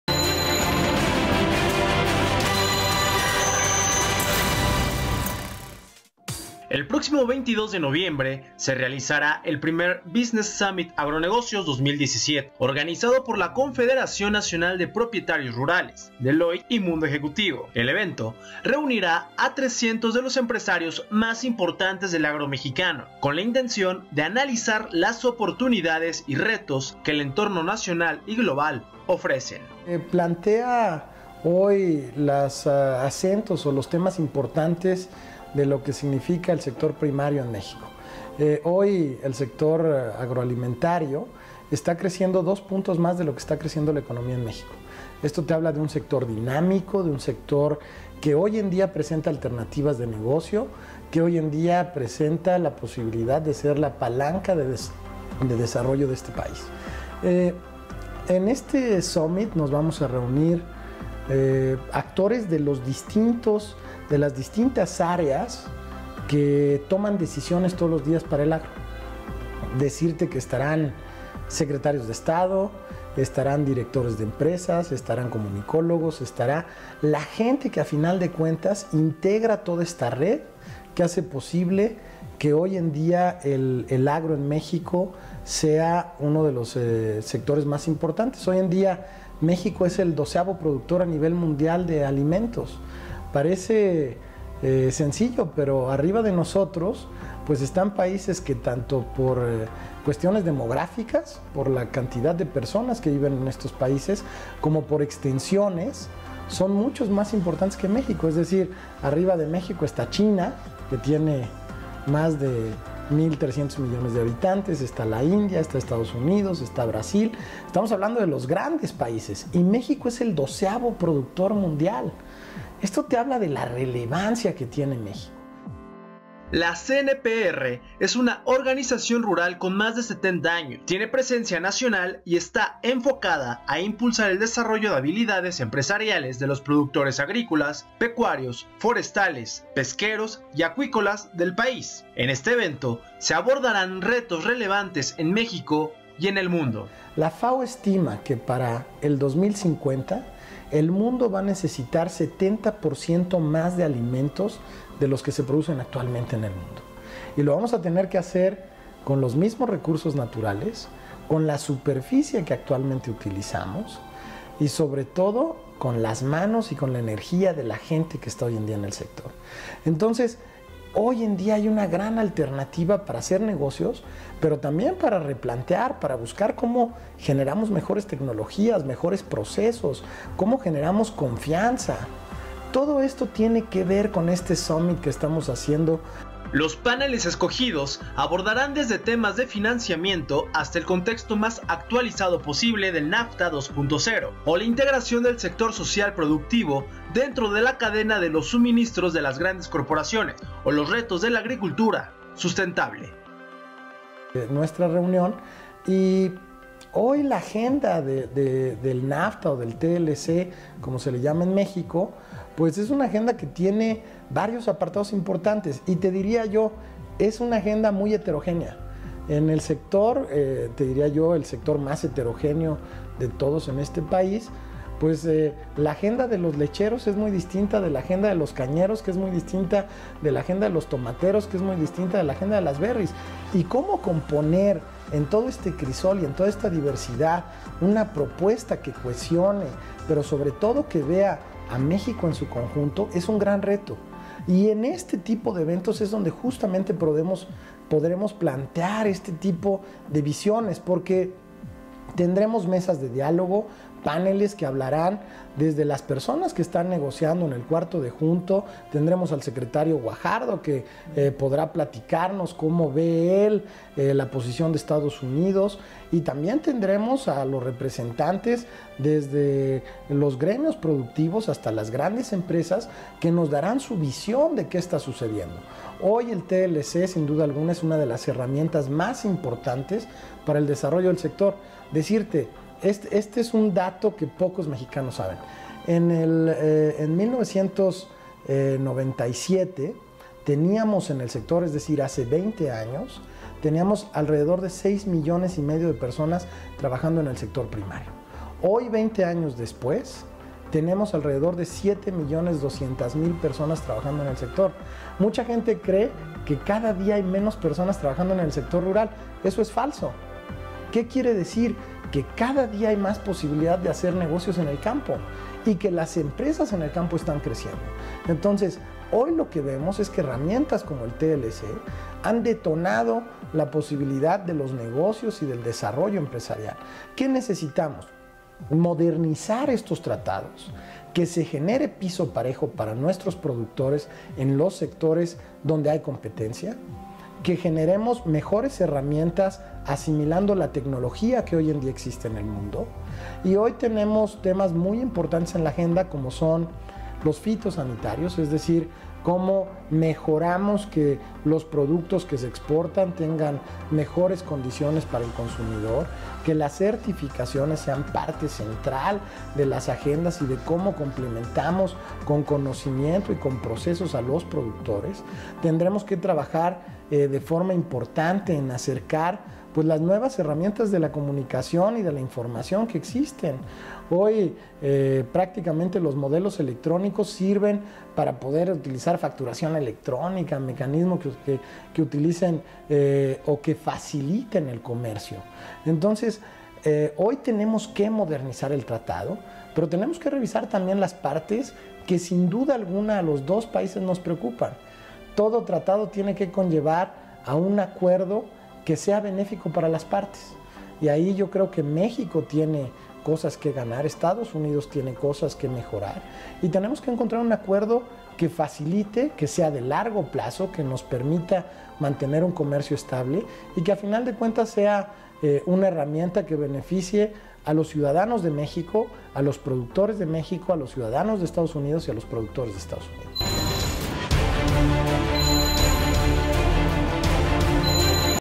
Foro Agronegocios 2017 análisis y retos de sector en México
Business Summit Agronegocios 2017 se llevará a cabo el próximo 22 de noviembre en salón Líderes Deloitte. El evento reunirá a 300 de los empresarios más importantes del agro mexicanos con la intención de analizar las oportunidades y retos que el entorno global ofrece.

El primer Foro Agronegocios 2017 es organizado por Grupo Mundo Ejecutivo, Deloitte y Confederación Nacional de Propietarios Rurales.